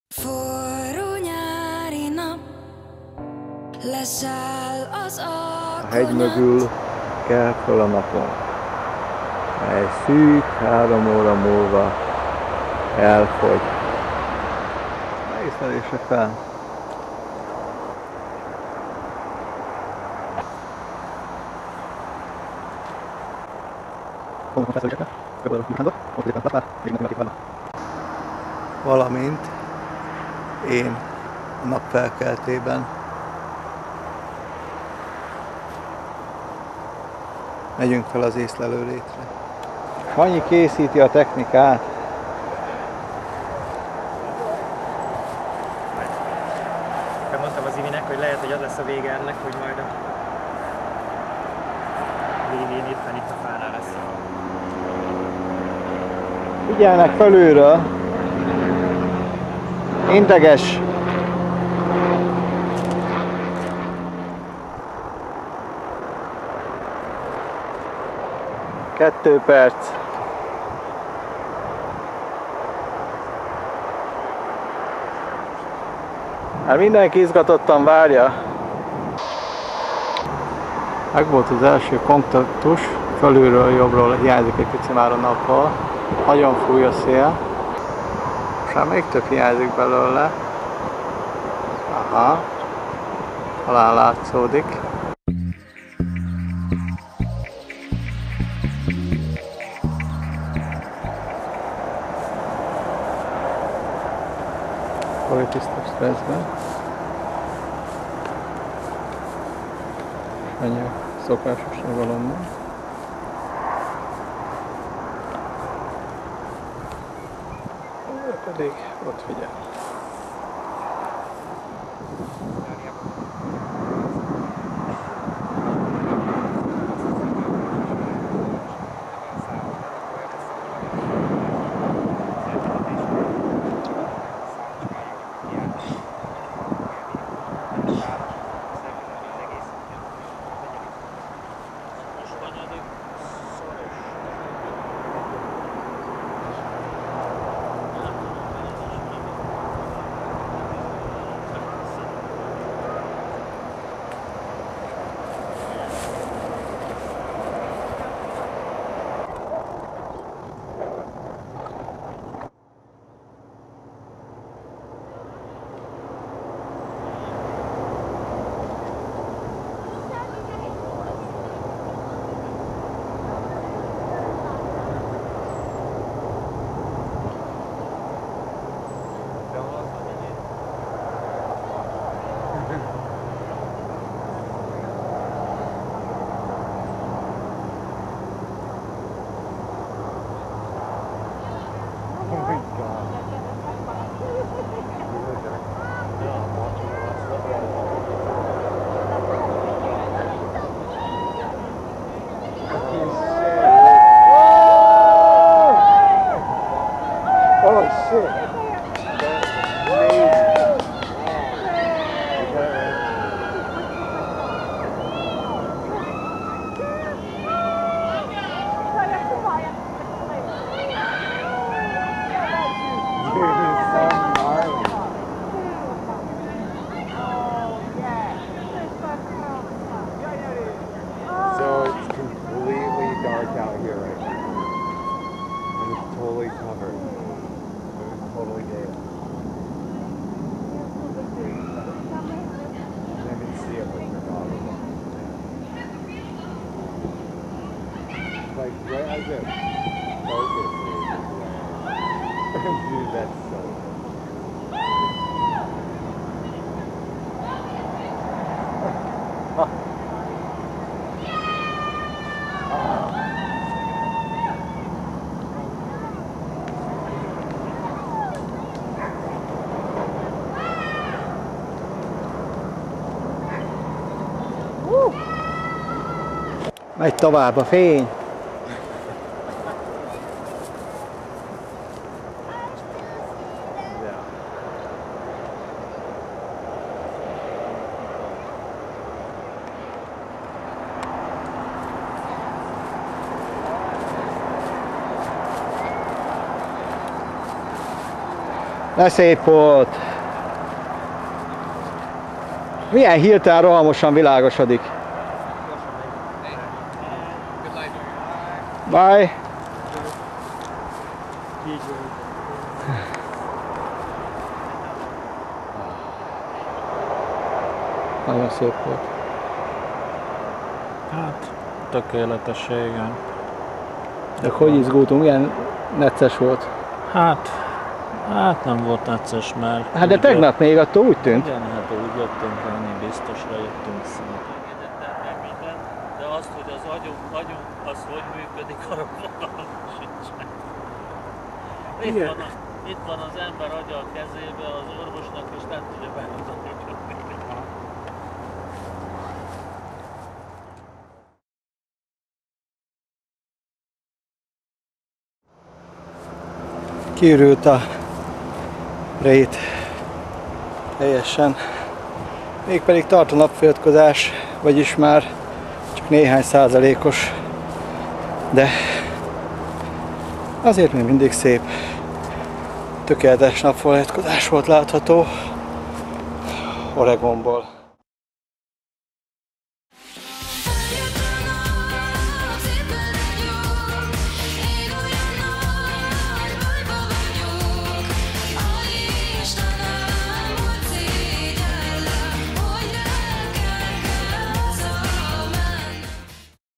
Total solar eclipse 2017 USA
A short movie of a Hungarian amateur astronomical expedition to Oregon, USA, 2017. Author János Bánfi